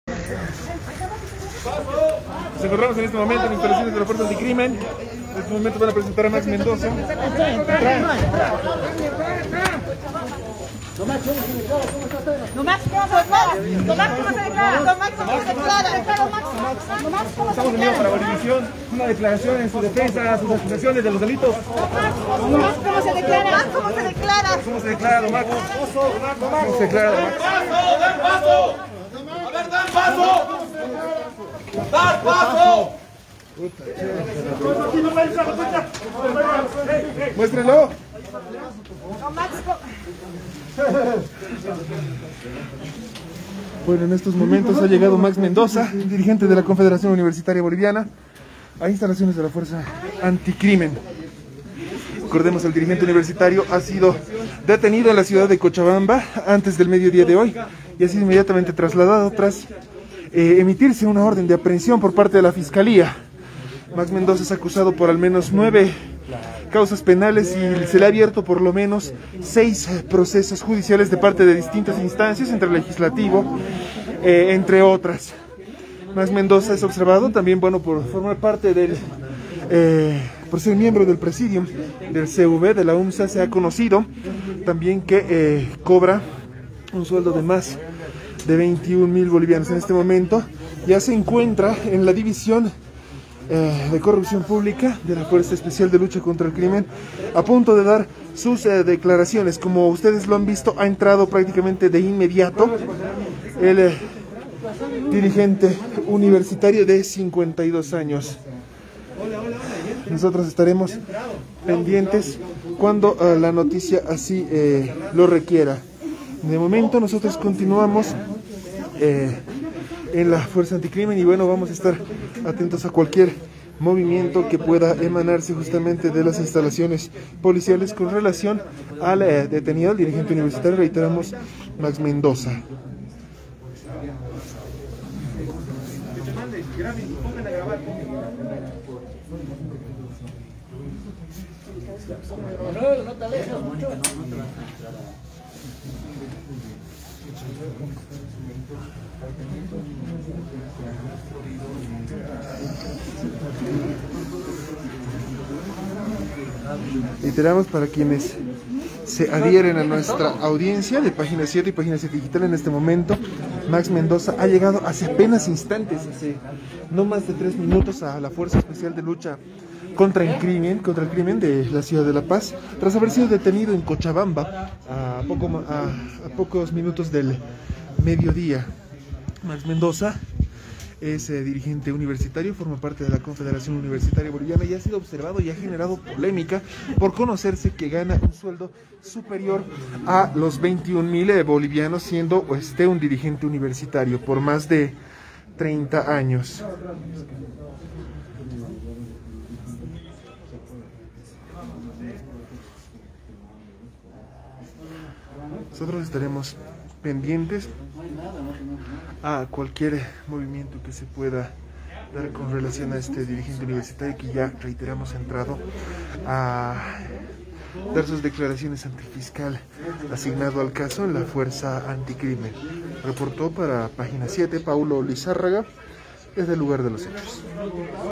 El dirigente universitario Max Mendoza llega a la FELCC de La Paz (Video: Página Siete)
El dirigente Max Mendoza llega a la FELCC de La Paz (Video: Página Siete)